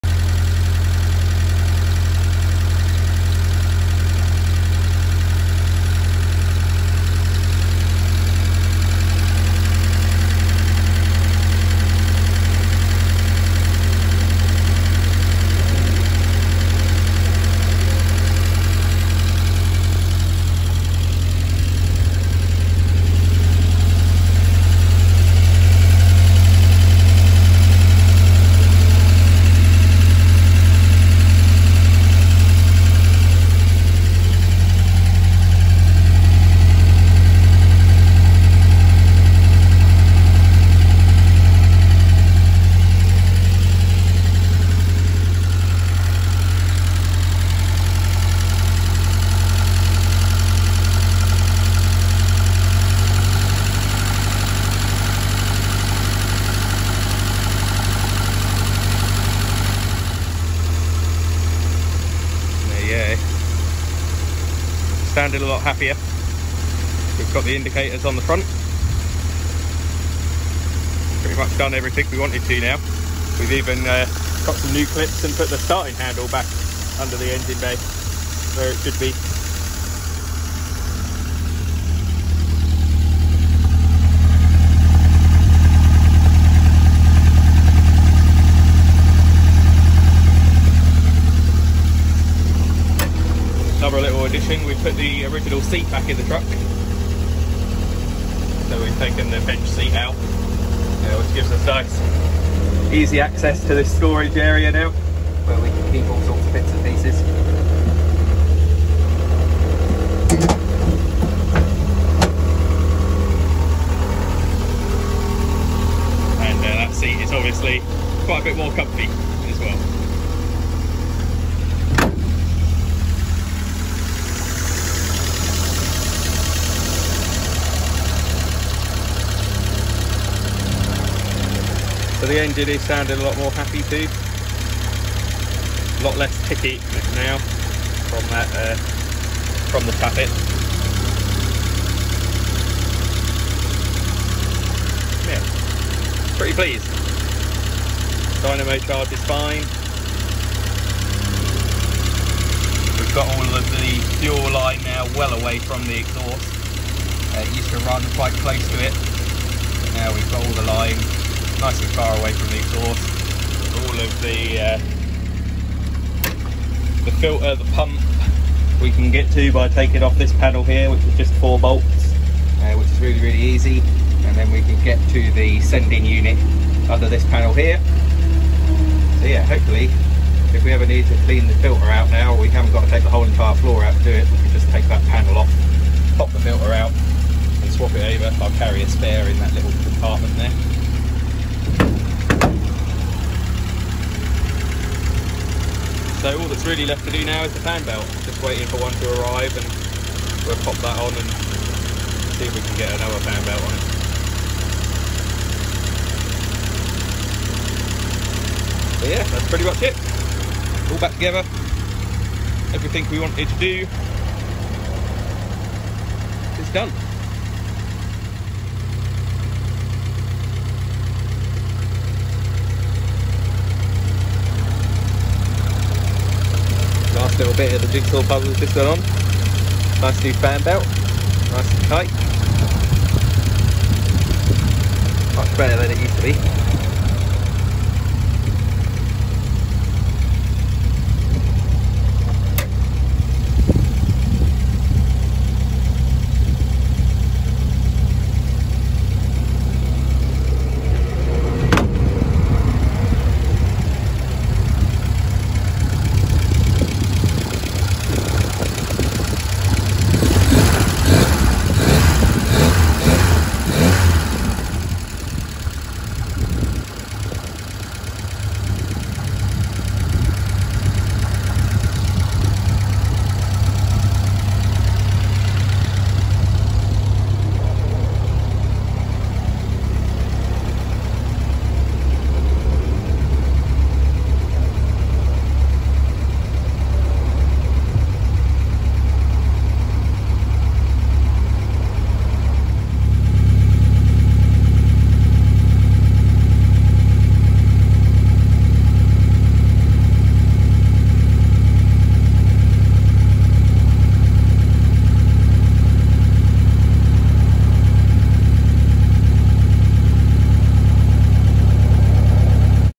1952 Fordson E83W - Repairs completed
The repairs to the Fordson E83W are now complete.

We have replaced;

Entire exhaust
Alternator
Regulator block
Fuel pump
Fuel lines and filter
Fan belt

And we’ve also adjusted the noises tappet which has reduced the ticking considerably.